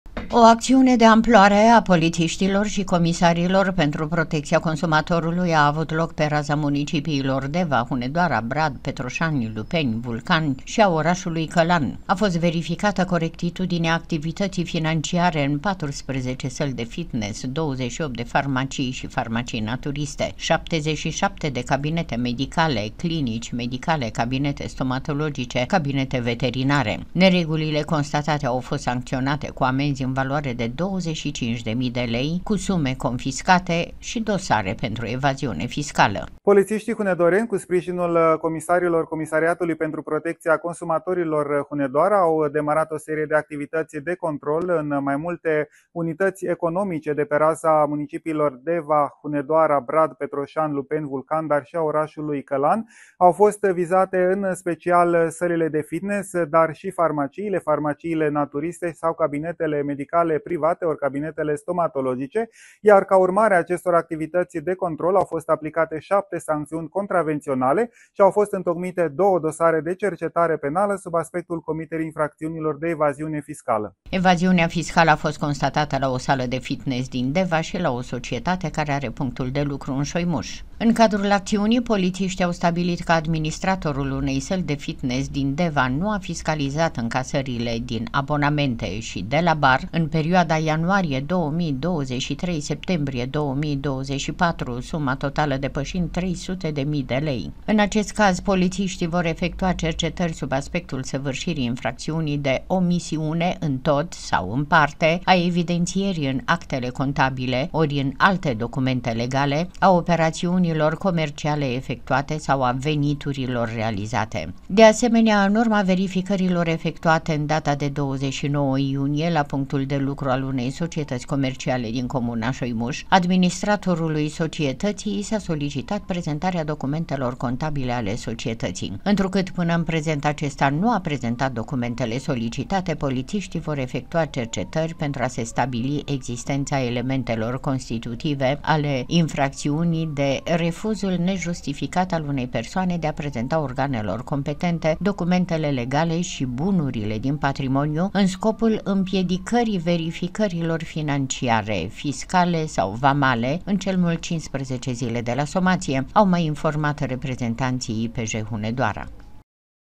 AMENZI, SUME CONFISCATE, DOSARE PENTRU EVAZIUNE FISCALĂ
ACȚIUNE A POLIȚIȘTILOR ȘI COMISARILOR PENTRU PROTECȚIA CONSUMATORULUI LA PESTE 100 DE AGENȚI ECONOMICI